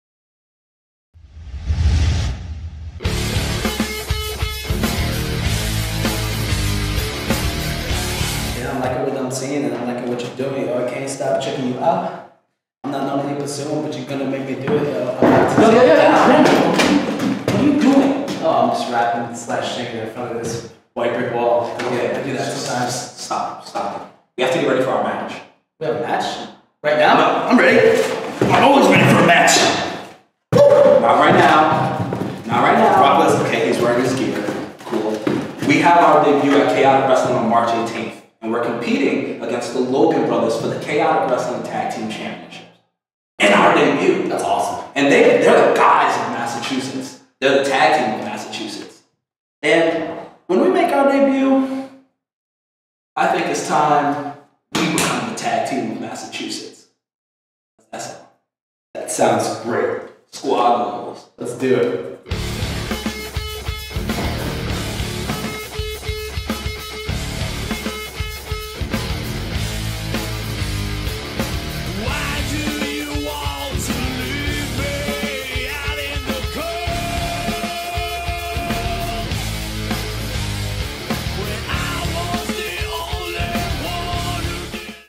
Milk Chocolate are ready for the Logans
Milk Chocolate are ready to take on the Logans for the Chaotic Wrestling Tag Team Championship Friday March 18th in Lawrence.